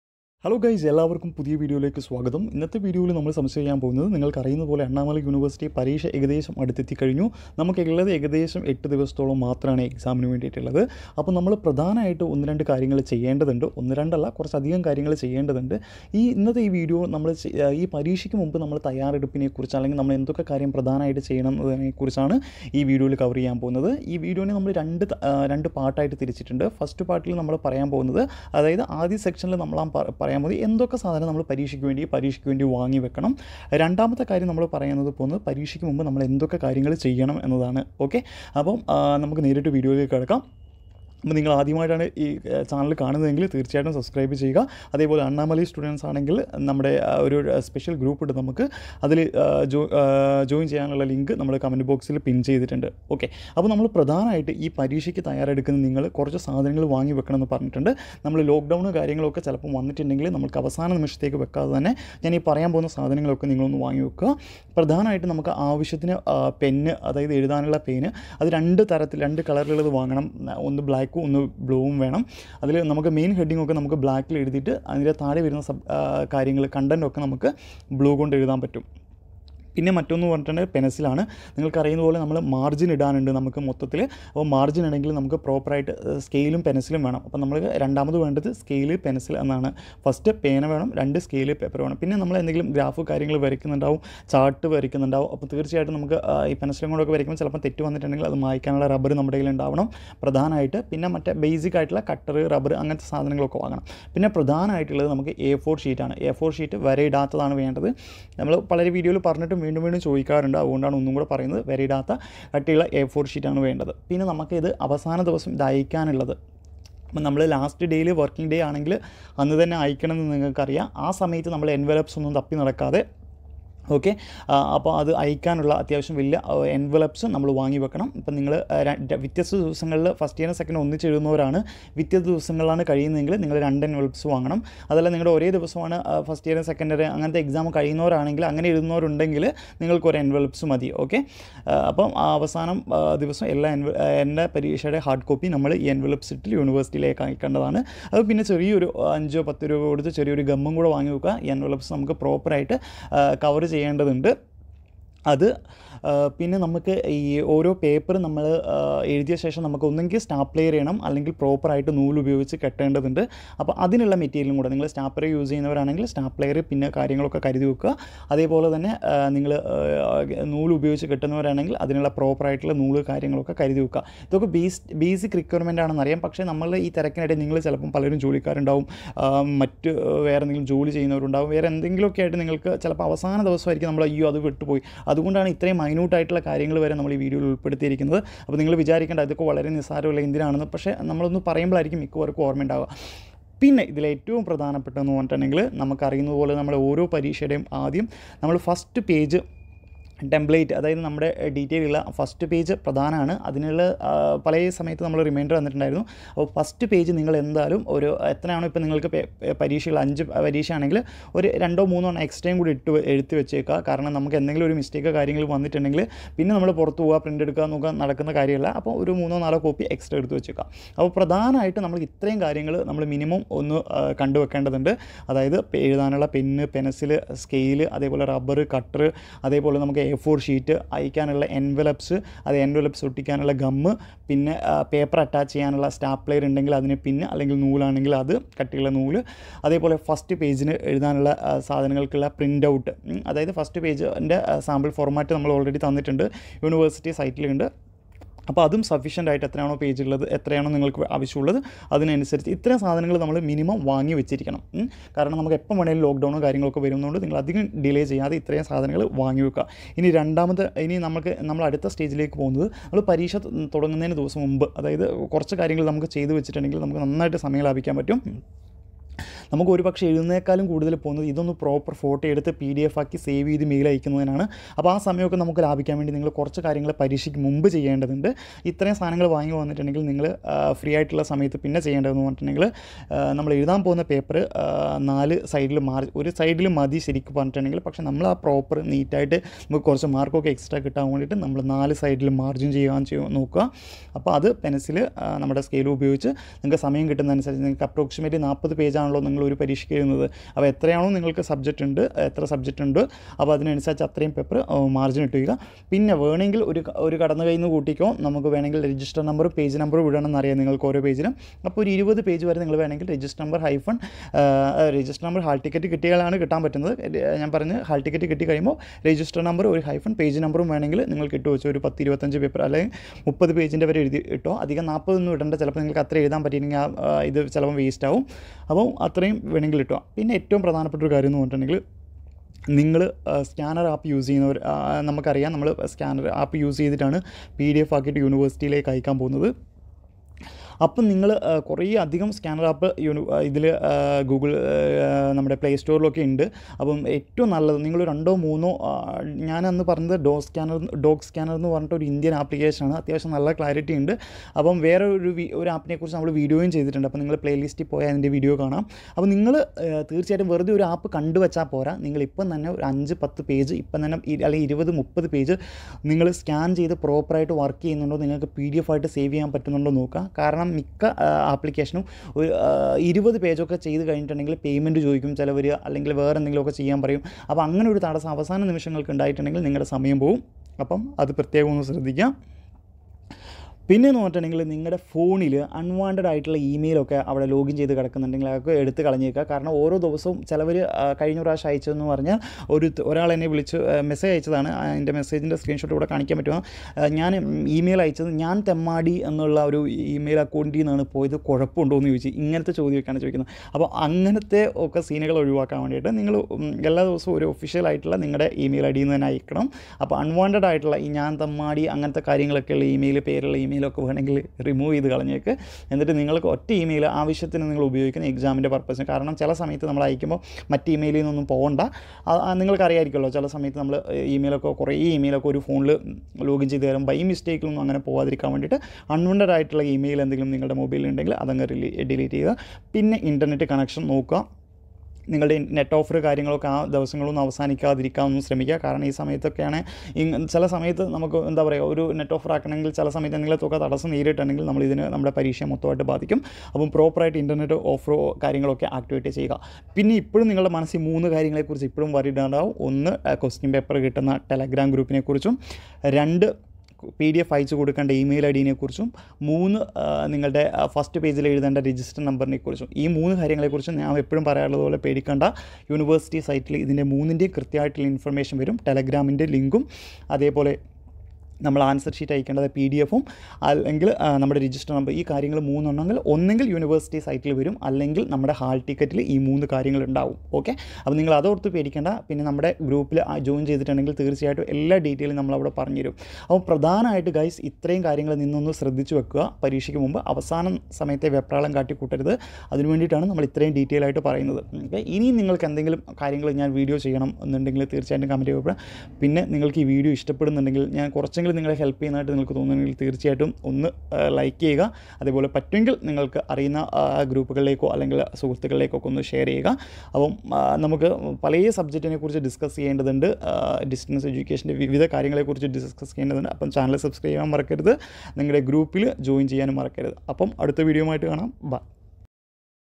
NOTEBOOK: THINKS TO DO BEFORE ONLINE EXAMINATION OF ANNAMALAI UNIVERSITY
This video explain must thinks do before examination to write exam proper manner and pass examination.